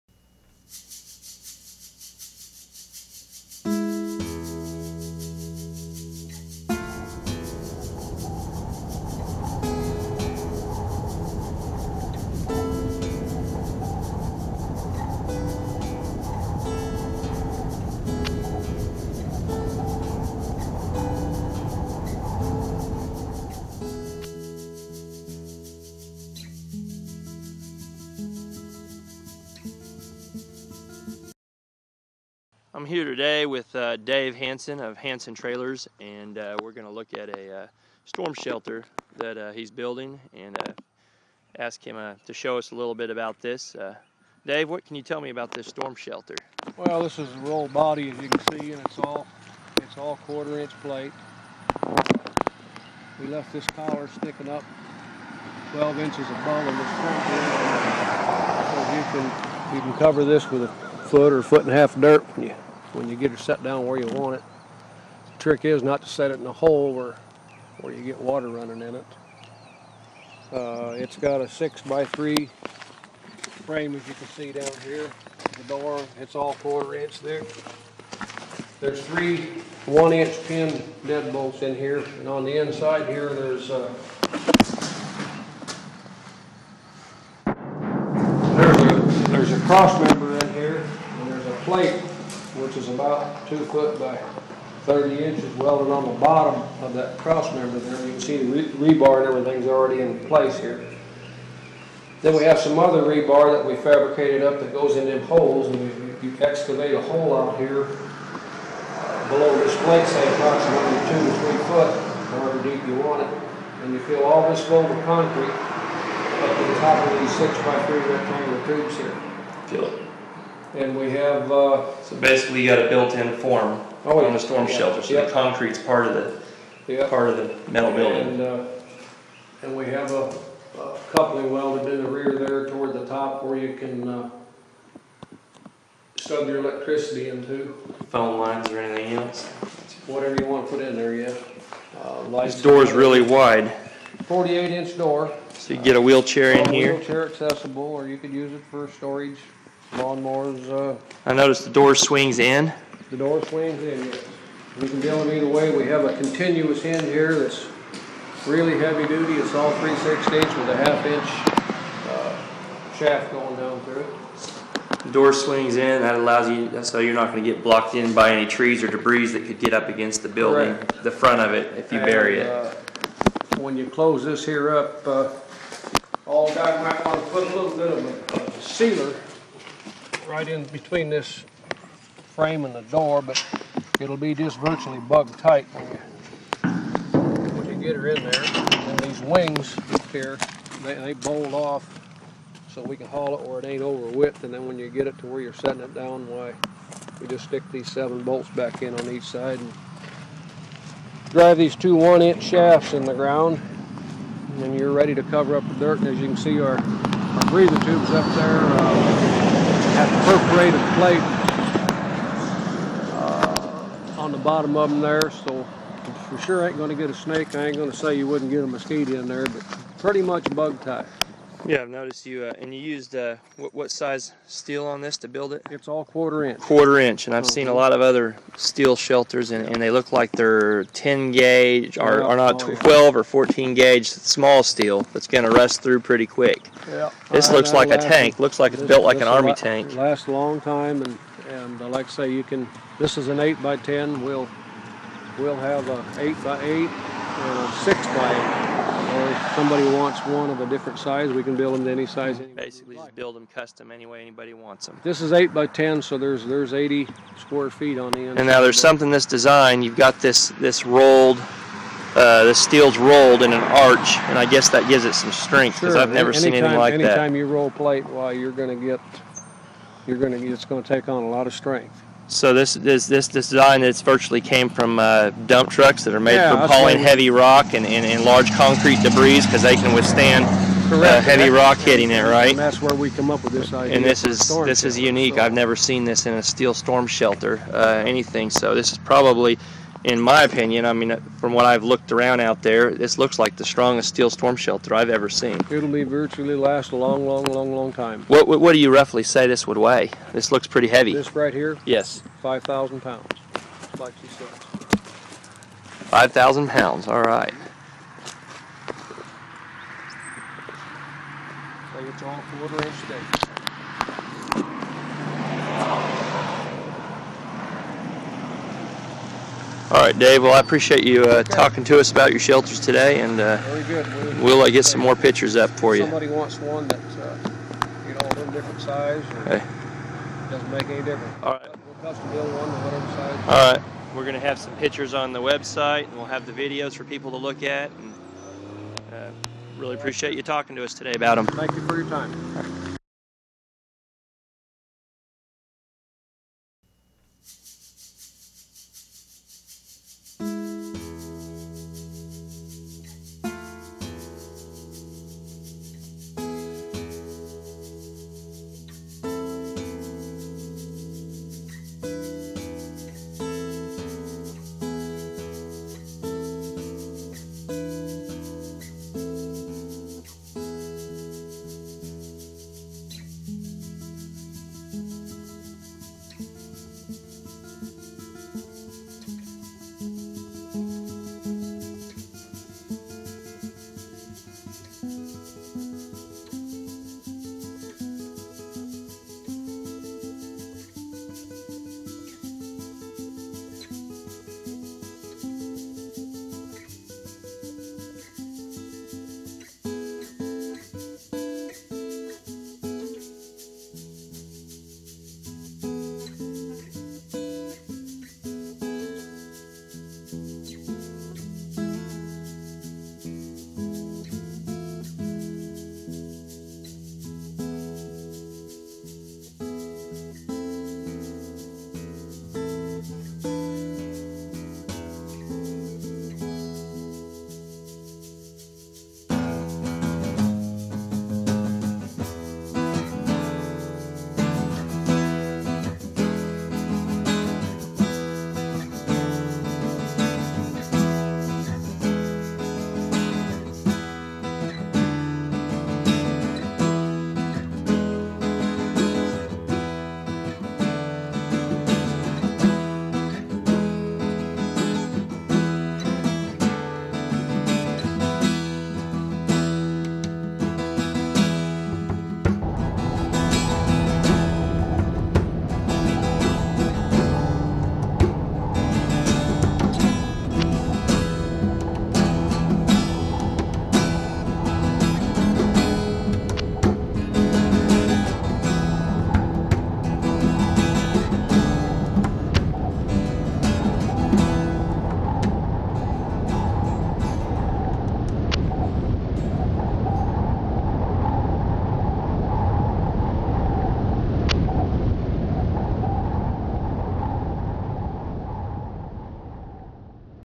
World's Strongest Steel Storm Shelter and Tornado Safe Room
Hanson Trailers manufactures the world's strongest steel storm shelters and safe rooms. Without a doubt you will be able to see that no one in the industry offers a storm shelter built like this. This shelter is built out of 1/4" Steel Plate and then it is rolled to give it superior strength with an arch. Everyone knows that an arch adds unsurpassed strength to any design. This unique rolled arch idea was taken from our dump truck designs that are made to move heavy rocks, debris and concrete. Please enjoy our video and look for more and we will show you the strongest storm shelter ever built. Some of the great advantages to our shelters is that they are wheelchair accessible and easy for elderly people to get in and out with no stairs. You won't see us using cheap, thin steel like other shelter companies that are using 10, 12 or 14 guage. With our 1/4" Steel Plate rolled into an arch to add even more protection our competitors will tell you everytime that we over build and we agree, but would you trust your family to anything less than the strongest safe room or Tornado Shelters available.

You might find cheaper shelters, but they will not offer the quality or safety  of Hanson storm shelters.

Standard Shelters include:
• 8' by 10' Storm Shelter or 80 square feet that's enough room for 16 people
• 48" wide door for easy access for wheelchairs or equipment
• 3 inside latches that 1" pin dead bolts just like a safe
• 1/4" thick steel plate skin
• Inlet for electricity and telephone for powering lights, radios, fans, etc.
• Heavy exterior locking clasp secures your shelter and valuables
• Continuous door hinge 3/16" with 1/2" center bar
• Ventilation Shafts non-crushable waterproof, snake proof, and bug proof
• 6" X 3" tube frame
• Exclusive rolled steel plate makes an arch design for the strongest shelter you can get!
• Floor is 6" X 3" tubing  frame and is a pre-made concrete form with rebar built in
• Built in cross members and a tie down plate to anchor the shelter even deeper